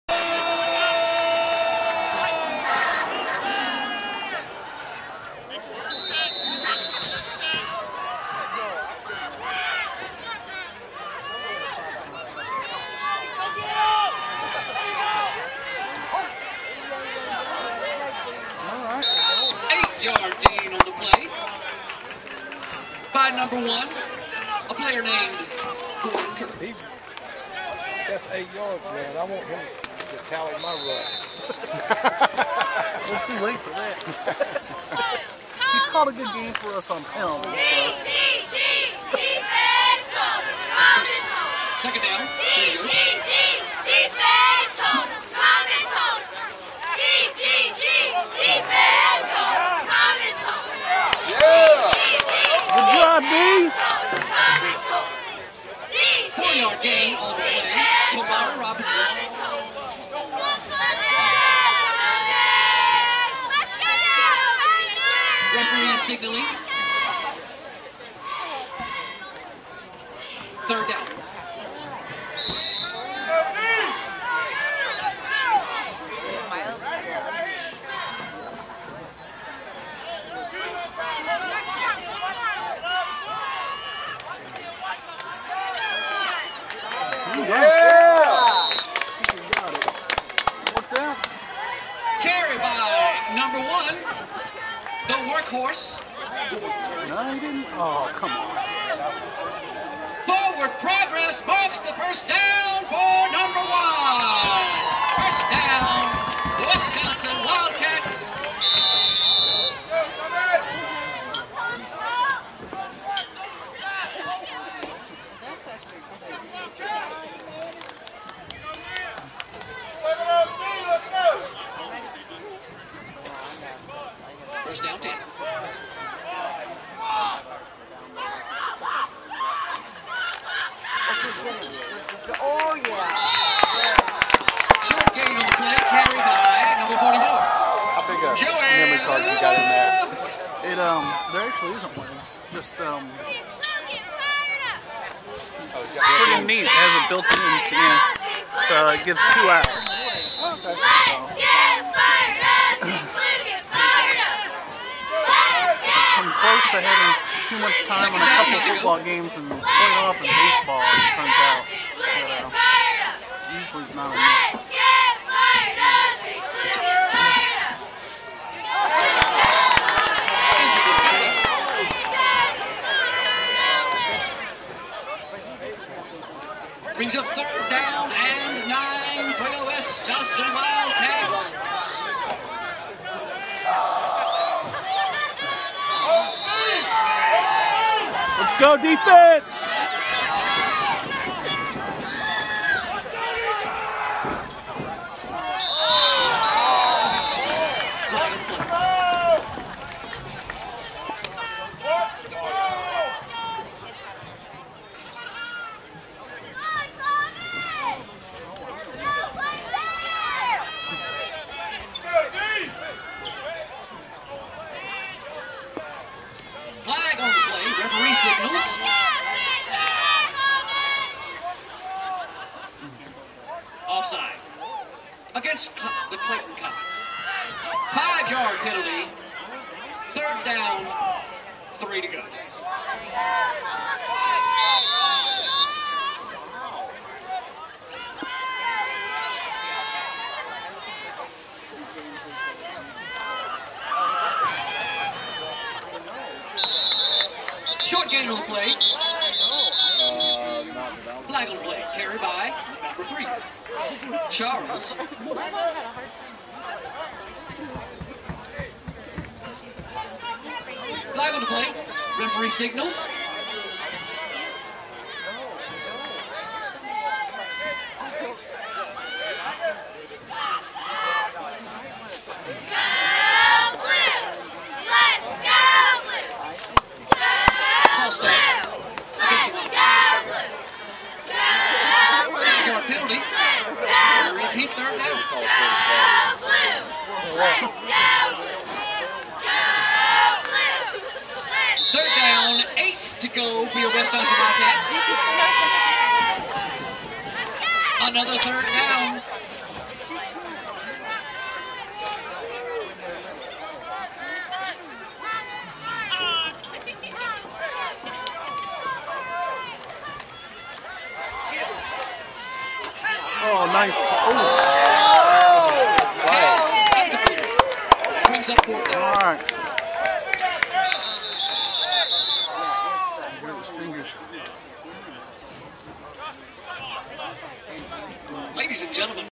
10/30 2014 Clayton Comets 18 @ West Johnston Wildcats 40 Junior Varsity Football game
John Ross Parrish leads the Comets on offense as Leiser is out with injury.  Freshman Quarterback Landen Leiser is wearing the #11 on JV & #19 on Varsity in a royal blue and/or white jersey.  The Clayton Comets compete in the NC 4-A Greater Neuse Conference.  Landen stands about 6'3 & weighs 175 pounds for the high school football team based in Clayton, North Carolina.  The Comets are coached by Randy Pinkowski, Anthony Barber, Allen West, Marc Rogers, Steve Moats, Matt Williams & Corey Blackmon.